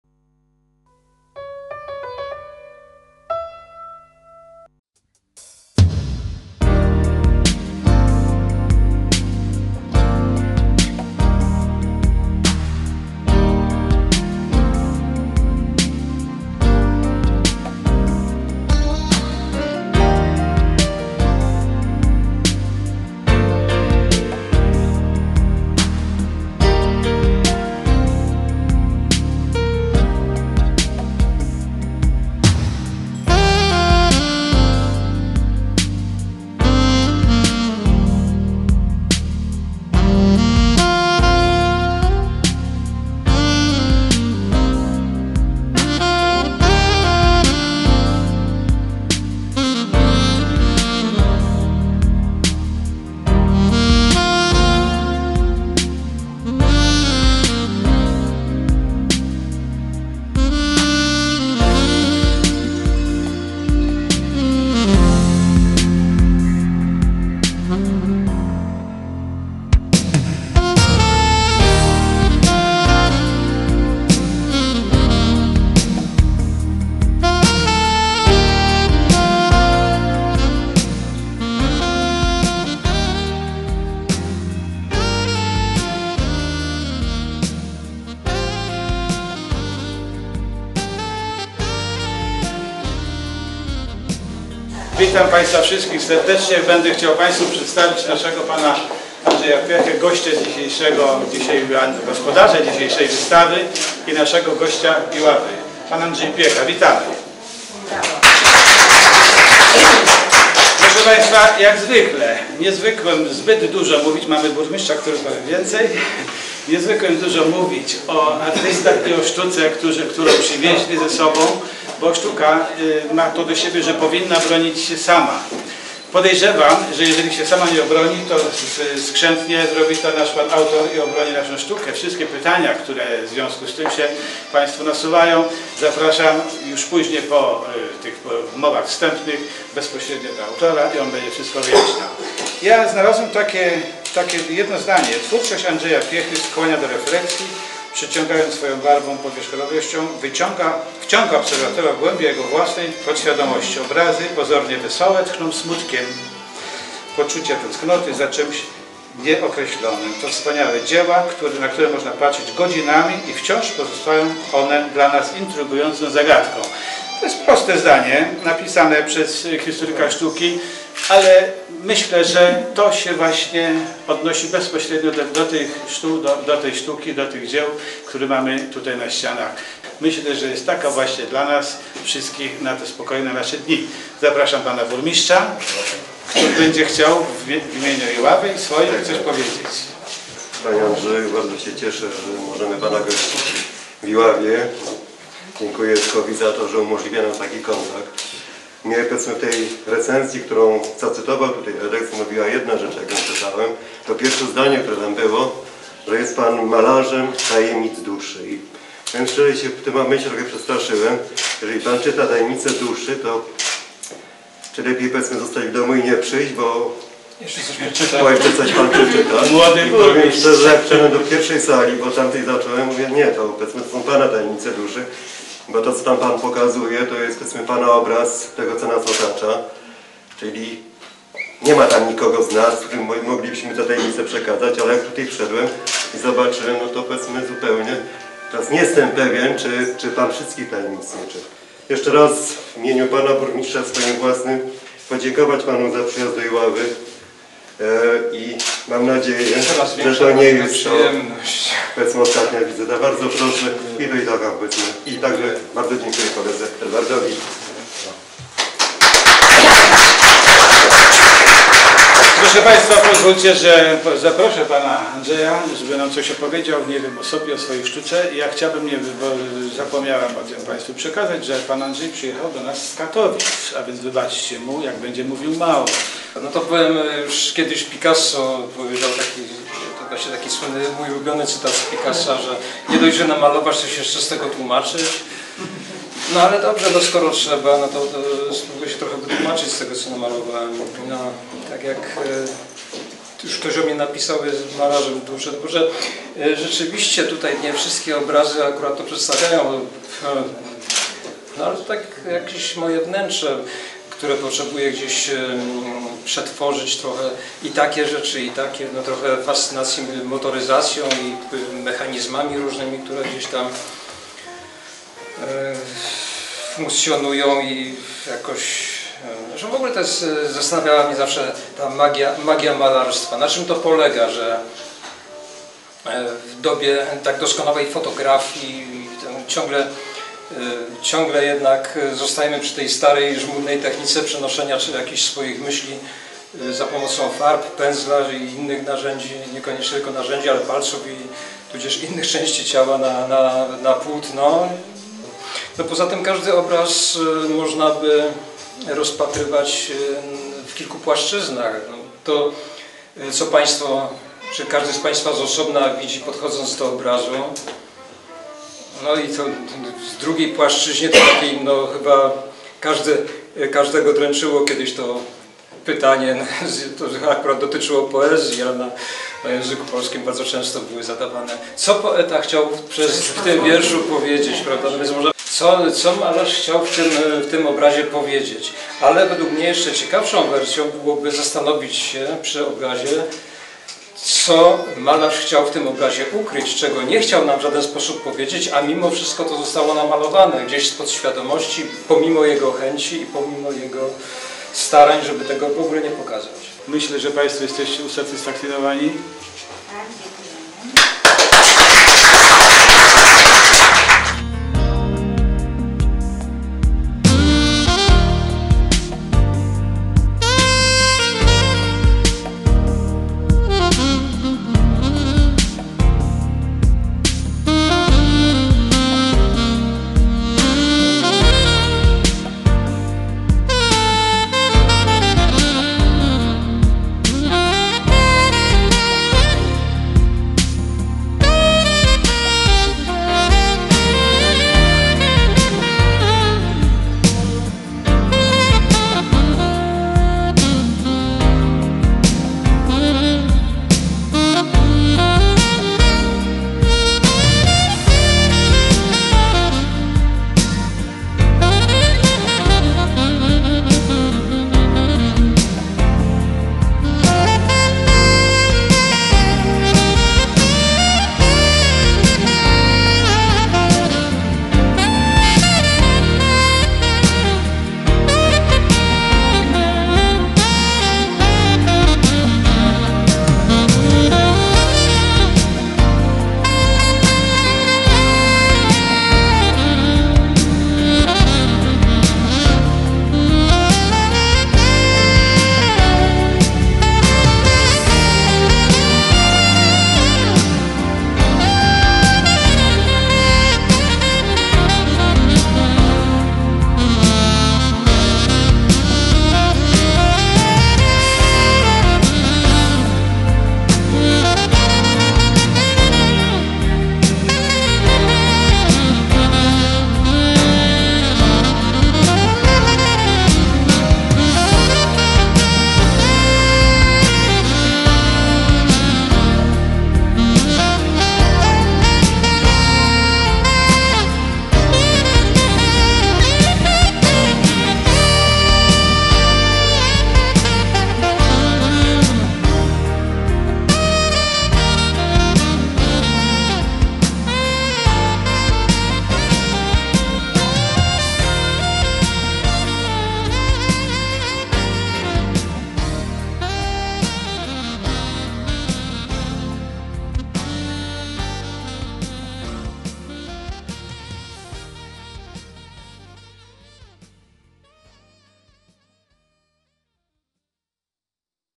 Andrzej Piecha.flv
Wernisaż wystawy prac Andrzeja Piechy w iławskiej Galerii "na wprost"